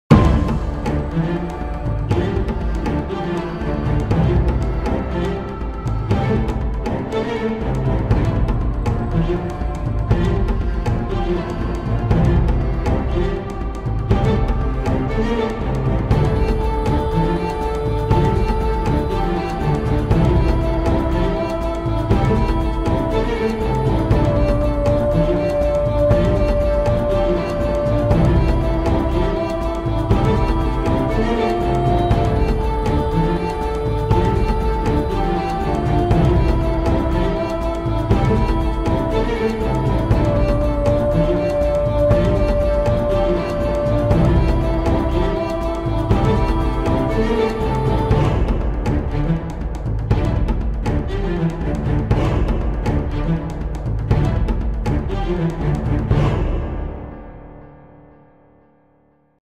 Deinoterium animation
This is Deinoterium animation from our next item 'Paleozoic Dino Zoo'. it have 5 animation.
1.Idle
2.Attack
3.Walk
4.Eat
5.Roar

If you want more information, please visit us !